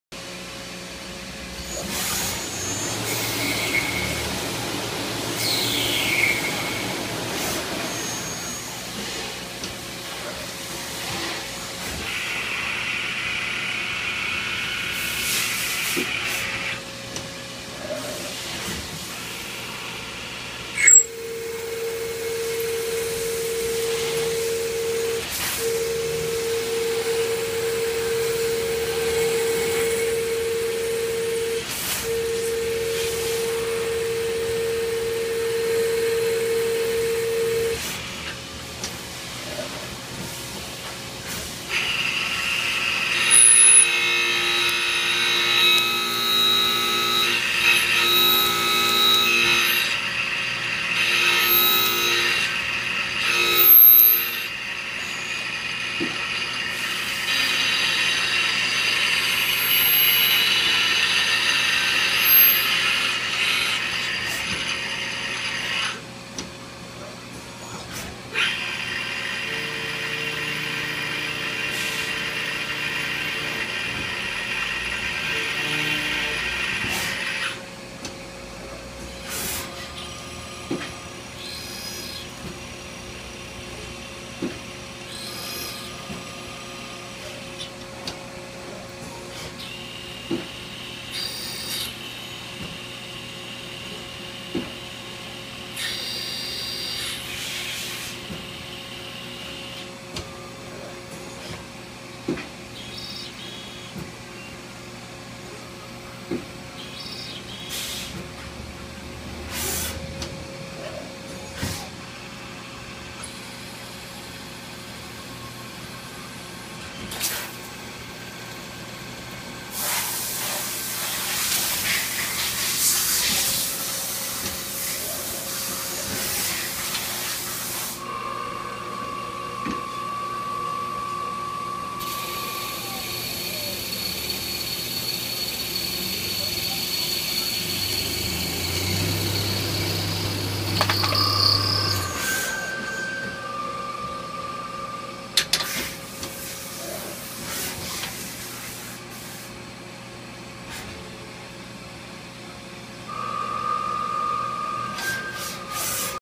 Cam with set screws, Brett Austin
A steel cam, material 1 3/4" lead alloy, approximate cycle 3:10. Made on a Mori-Seiki NLX2500-SY Nov. 2014